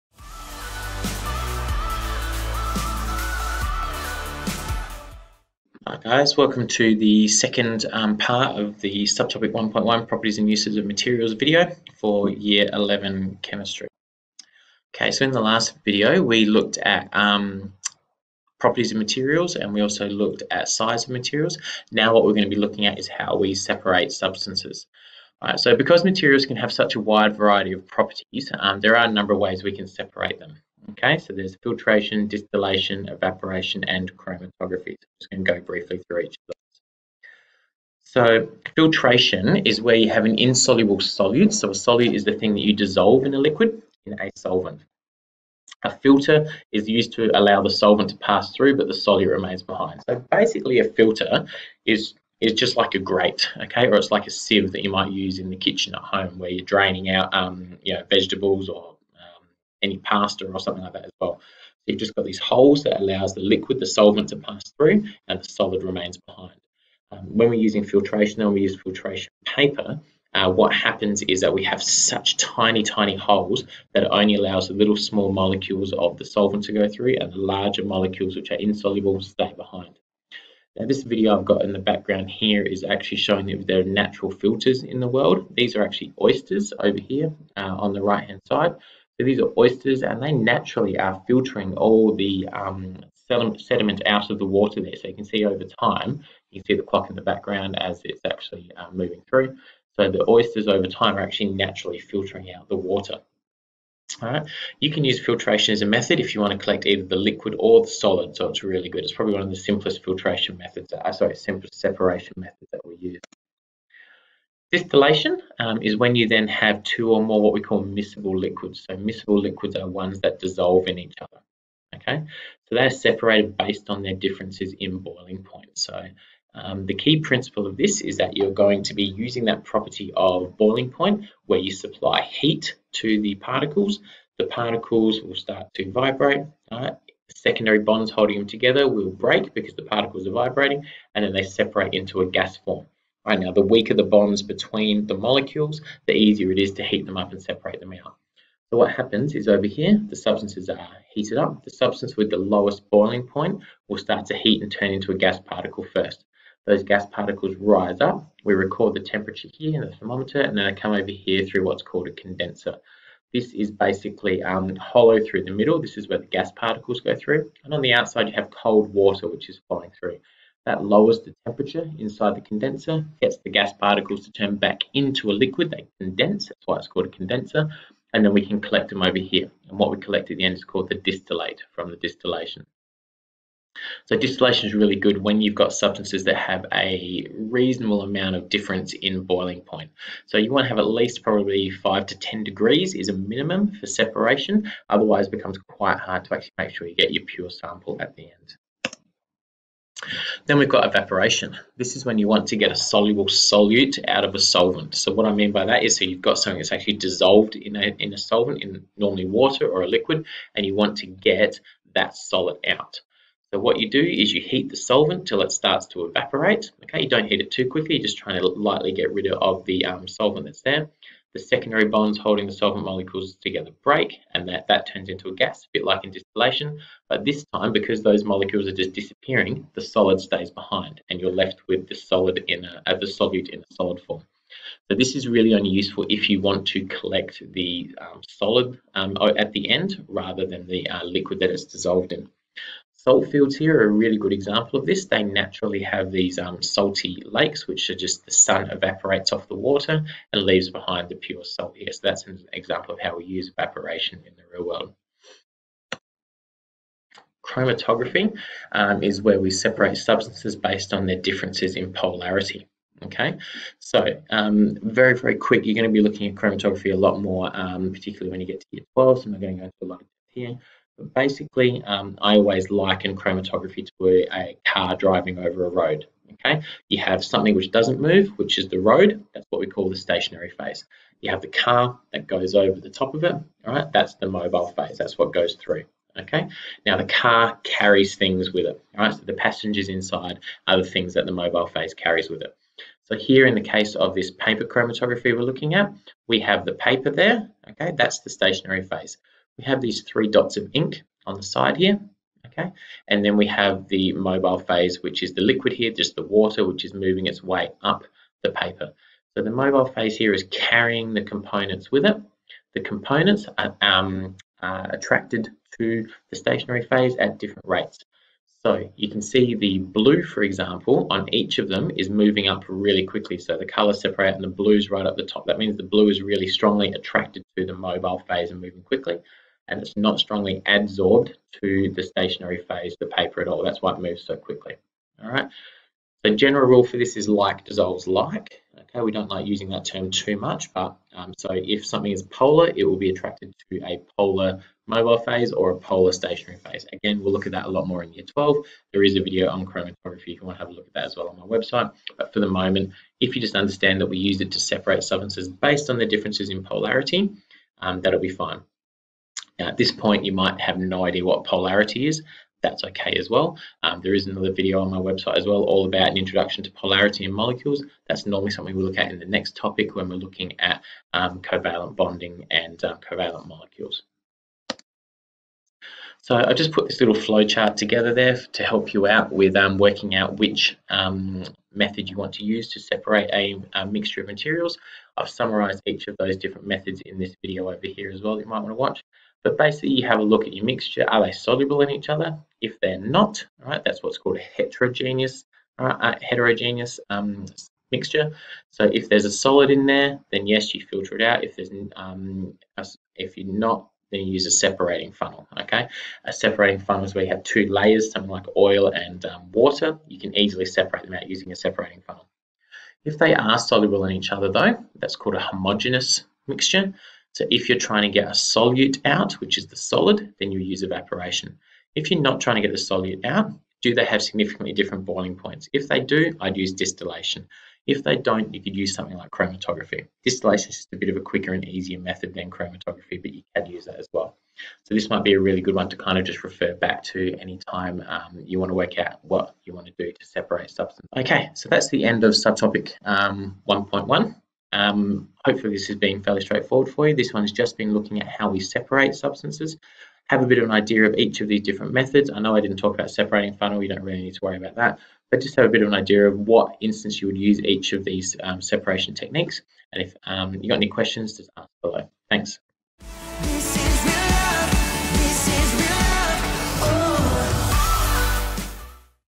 Materials & Their Atoms Part 2
The second part of the Materials and Their Atoms PowerPoint for Stage 1 SACE Chemistry. Covers slides 14-19.